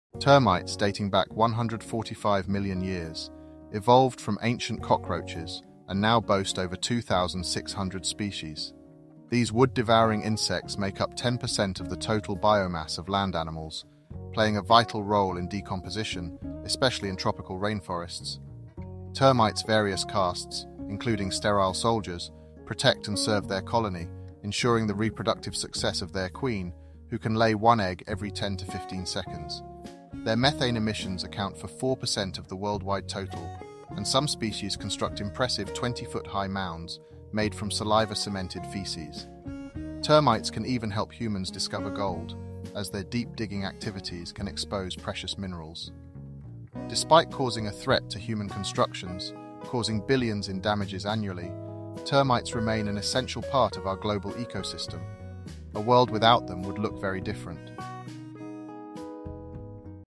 Termite Trivia
Embark on an extraordinary exploration of termites, the unsung heroes shaping our planet. While humans believe we're the most significant species, eusocial insects like termites play critical ecological roles.

Dating back 145 million years, termites evolved from ancient cockroaches and now comprise over 2,600 species. These wood-consuming insects account for 10% of all land animal biomass, crucially decomposing matter in tropical rainforests. Termites' caste system, including sterile soldiers, serves their colony by ensuring the queen's reproductive success, who lays an egg every 10-15 seconds.

With a global influence, termites contribute 4% to worldwide methane emissions, affect modern tree evolution, and create red-tropical clays. As nature's master architects, some species build towering 20-foot mounds from saliva-cemented feces. Termites even assist in gold discovery due to their deep-digging activities.

Despite causing billions in damages annually, termites remain indispensable to our global ecosystem—a world without them would be unrecognizable.